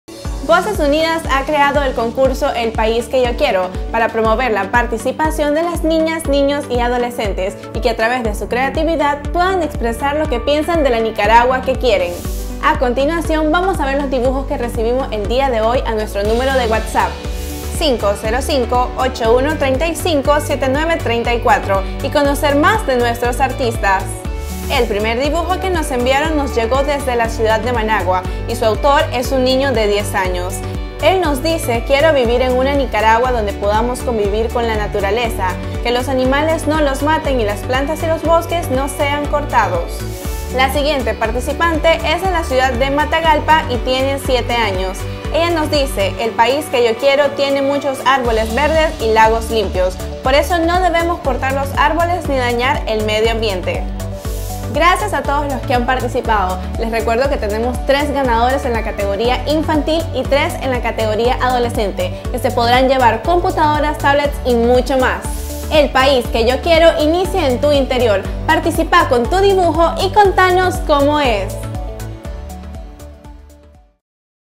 Concurso Nacional
🇳🇮🖍 ElPaísQueYoQuiero | ¿Qué necesitas para participar? Una hoja, lápices de colores y mucha imaginación para expresar cómo es la Nicaragua que querés. Vos podes ser el afortunado ganador.

📆 ¡Atentos! Tienen hasta el 25 de septiembre para participar.